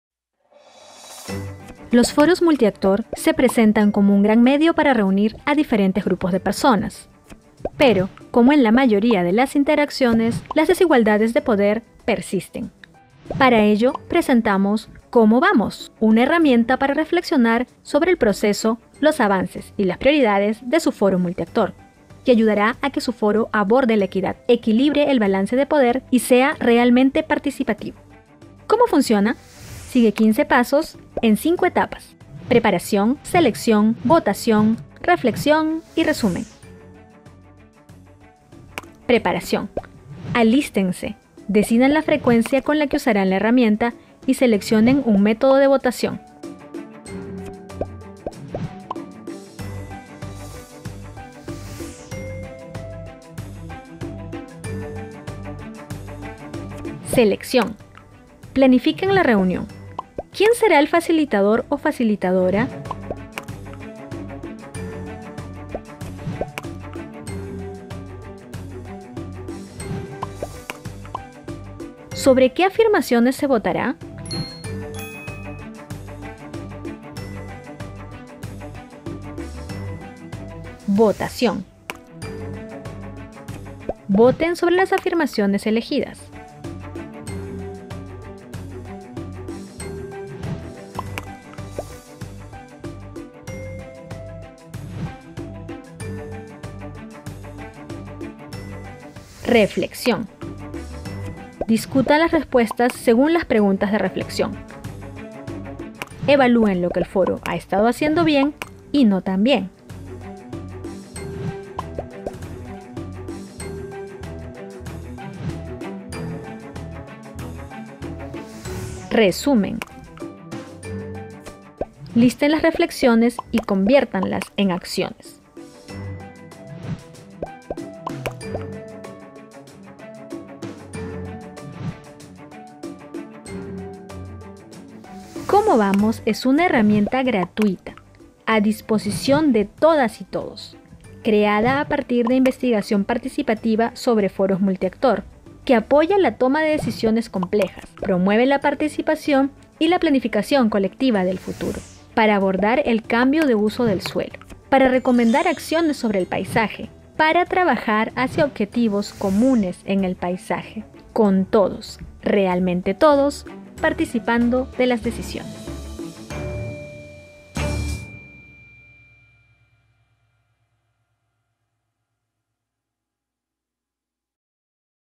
Una breve guía para implementar ¿Cómo vamos?
Conoce en este video los pasos necesarios para la implementación de ¿Cómo vamos? Una herramienta para reflexionar sobre el proceso, el progreso y las prioridades de su foro multiactor.